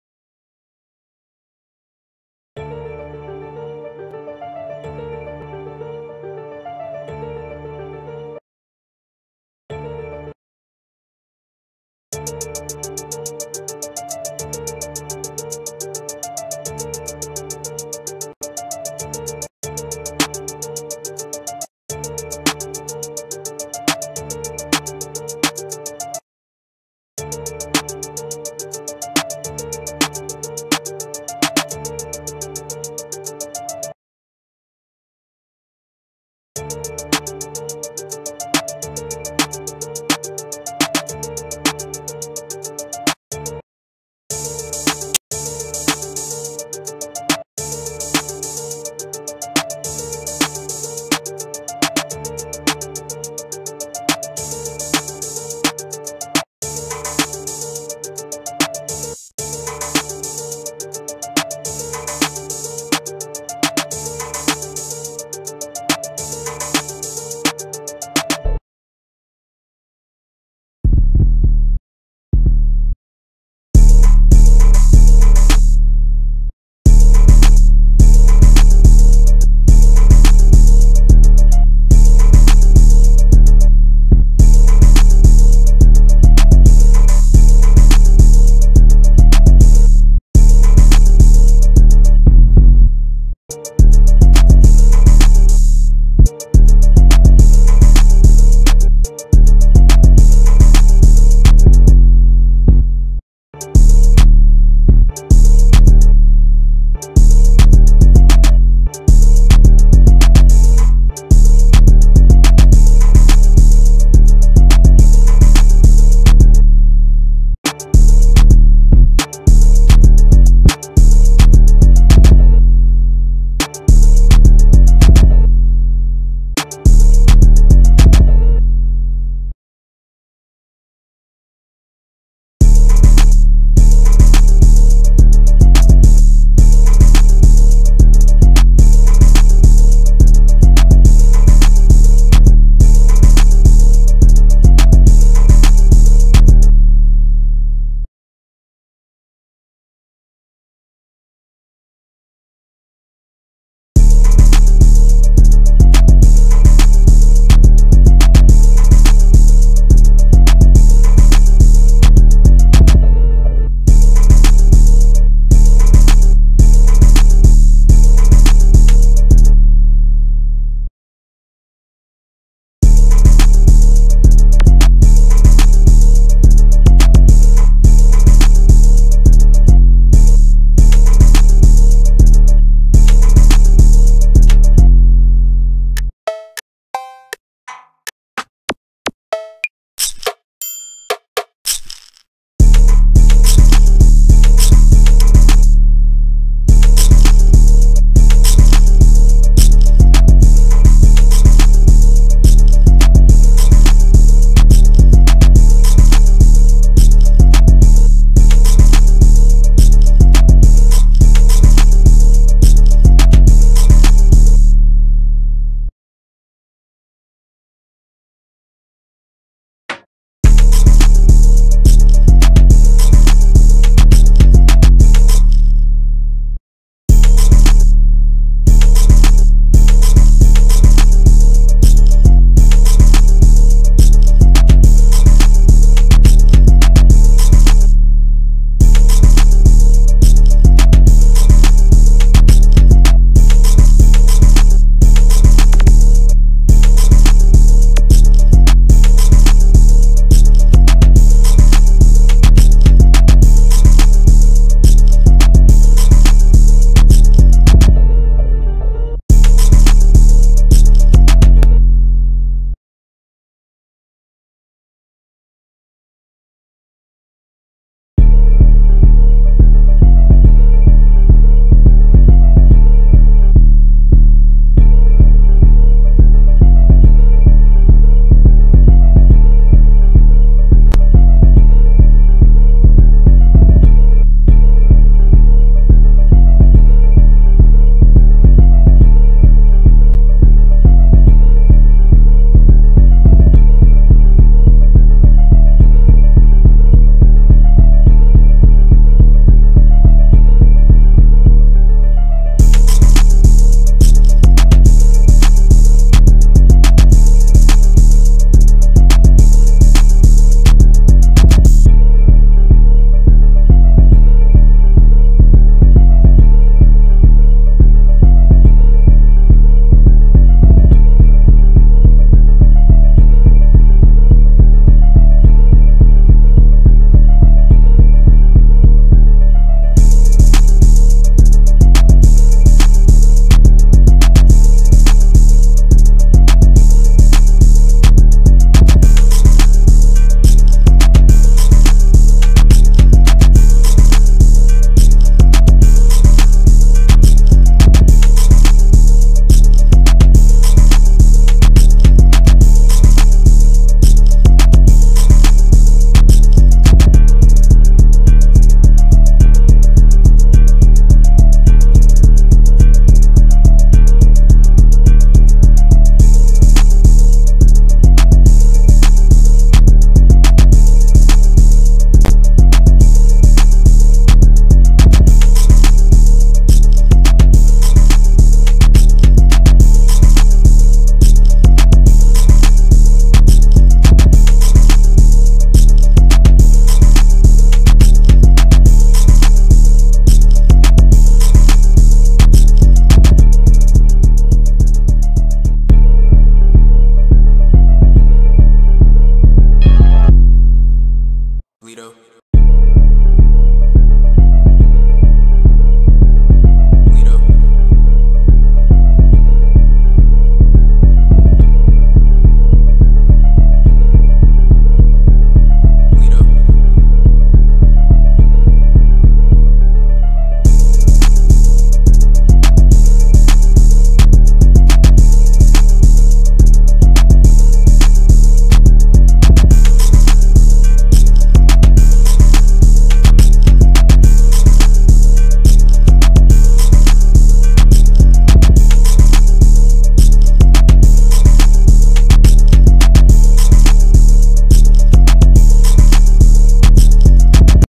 How to make beats for Lud Foe and Spinabenz
Leave any suggestions for future videos in the comments! All feedback is greatly appreciated.

The beat in this video is owned by ME and is not permitted to be used for any reason without contacting me.